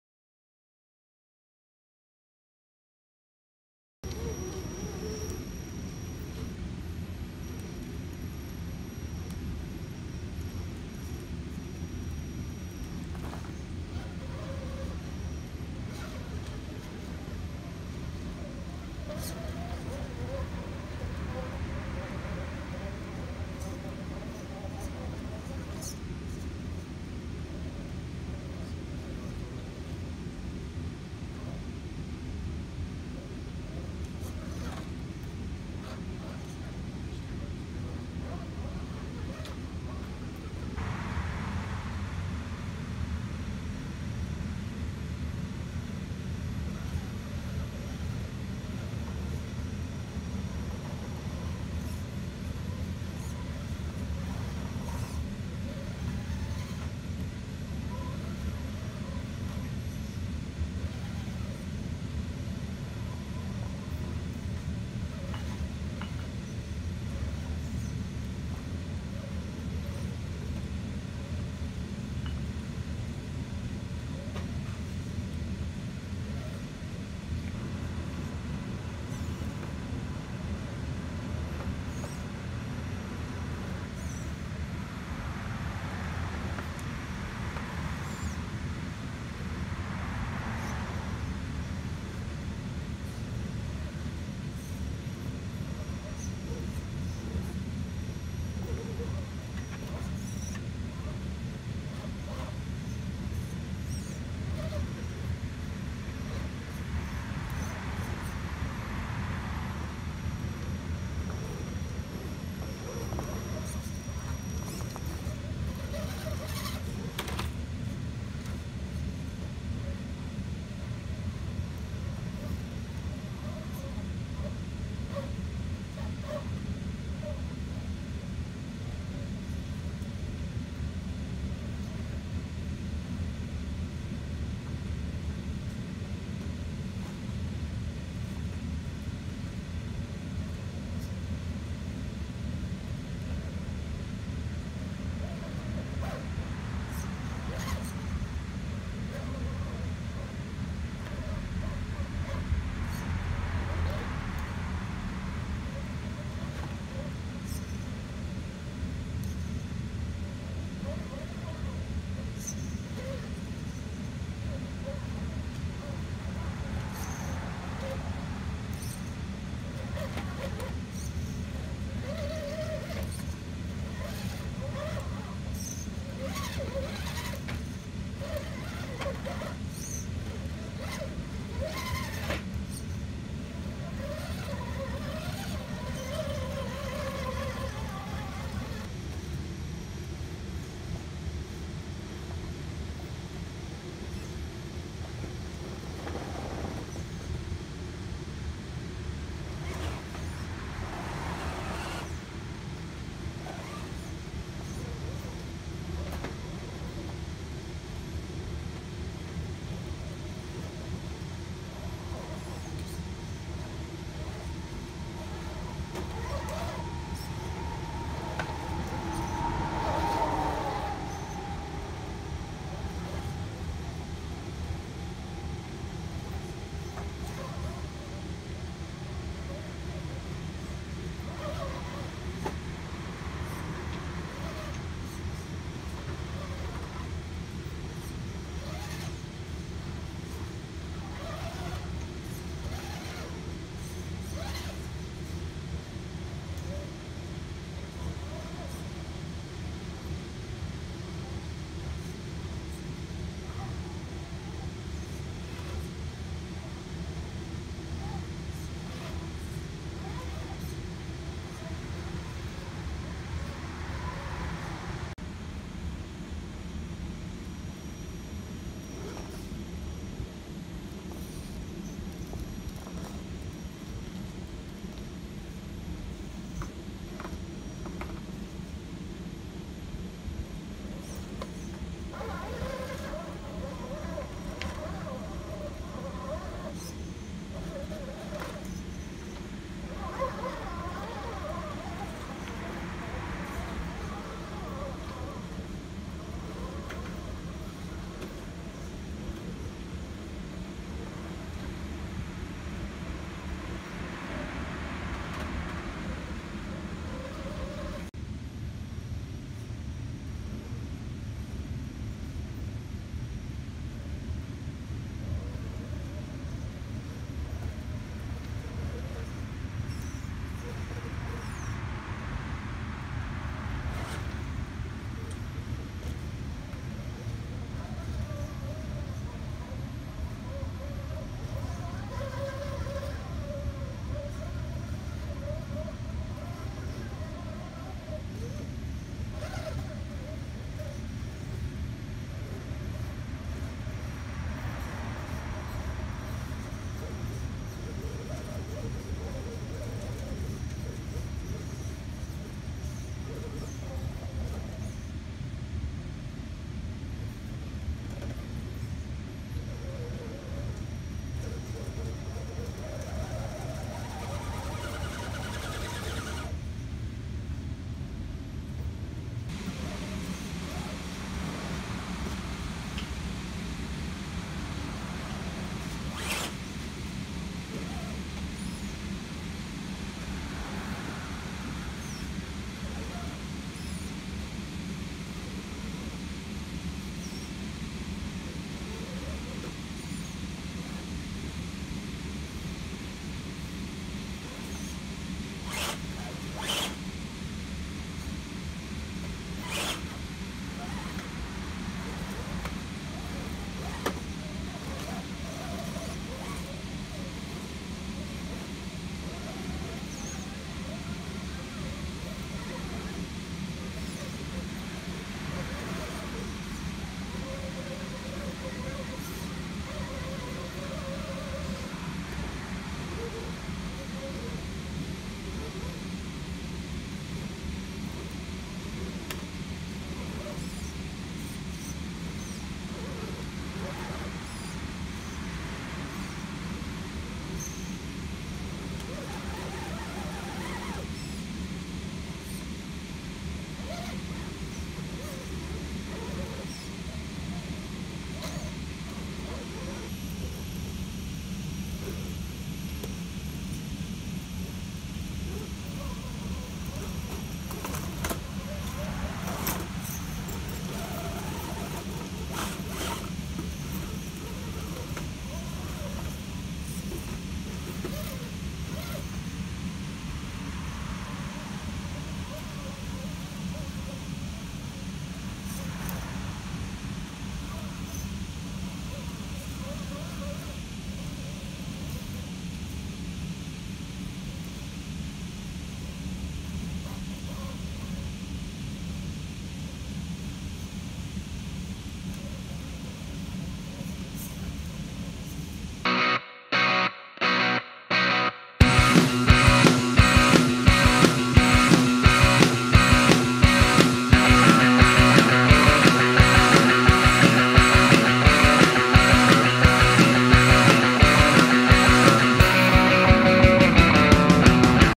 RC Rock Crawler Adventure
My dad and I took the crawlers and headed out to have some fun.